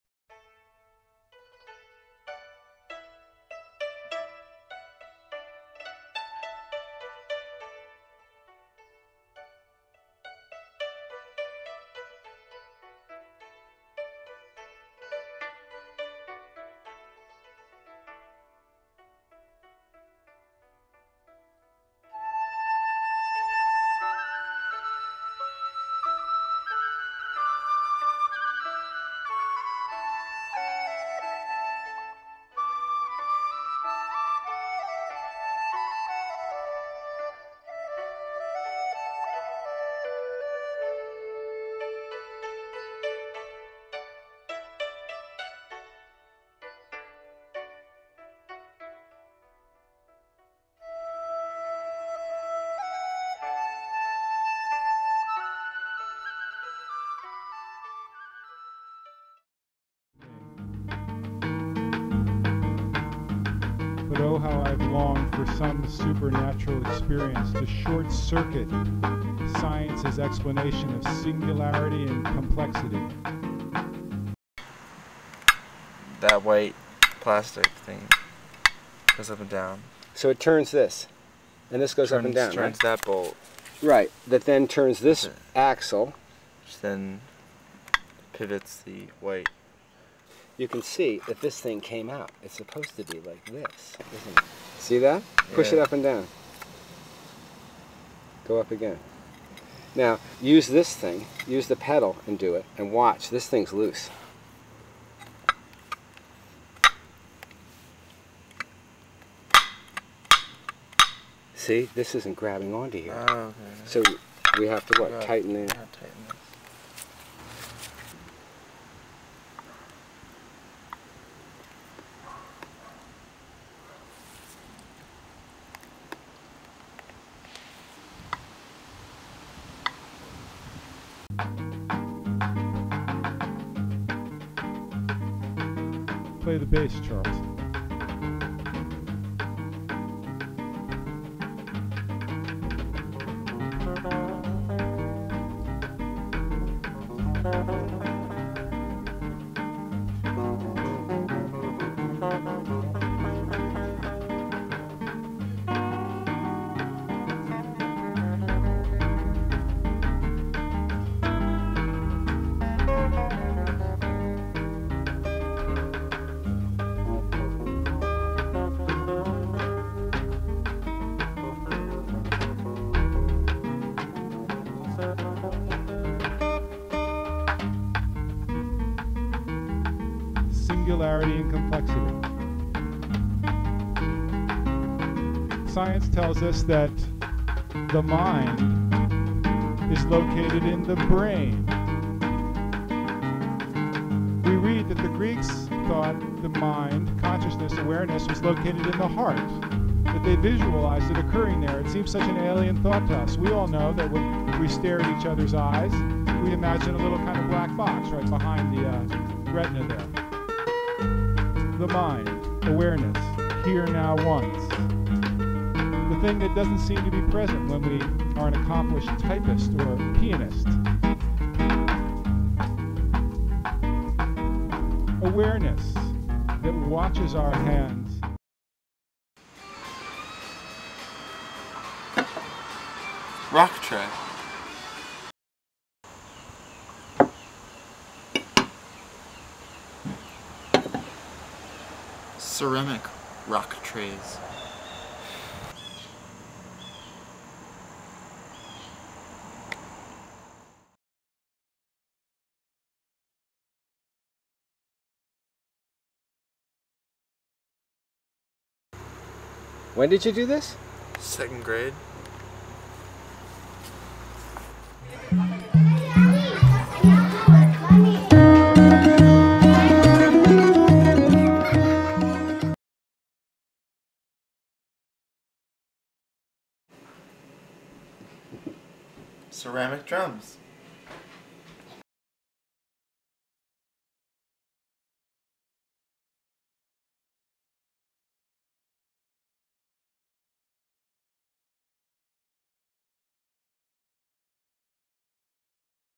Perry Mortimer - Ceramic Artist
An afternoon visit with Perry Mortimer, who began his exploration of clay and sculpture in the second grade. The pedal that controls the electric pottery wheel malfunctioned and Perry's immediate decision to dismantle and fix it impressed me as much as his enthusiasm for all things ceramic.